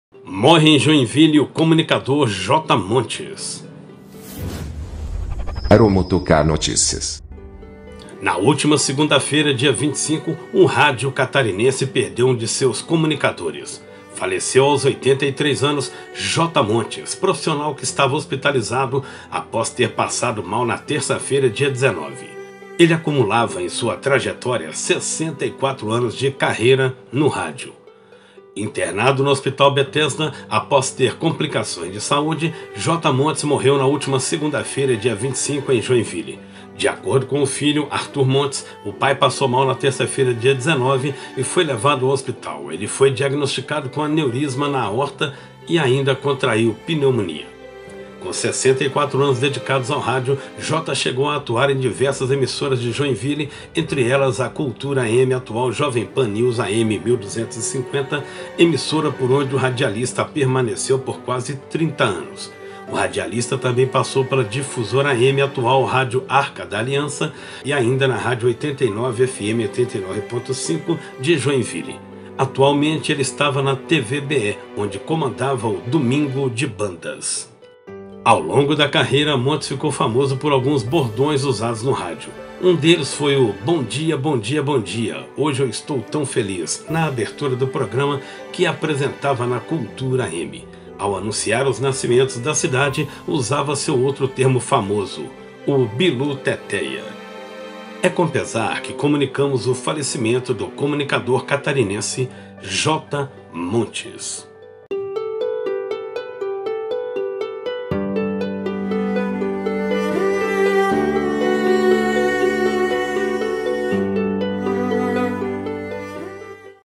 (LUTO NO RÁDIO) MORRE O RADIALISTA JOTA MONTES
Na última segunda-feira (25) o rádio catarinense perdeu um de seus comunicadores. Faleceu, aos 83 anos, Jota Montes, profissional que estava hospitalizado após ter passado mal na terça-feira (19). Ele acumulava em sua trajetória 64 anos de carreira no rádio.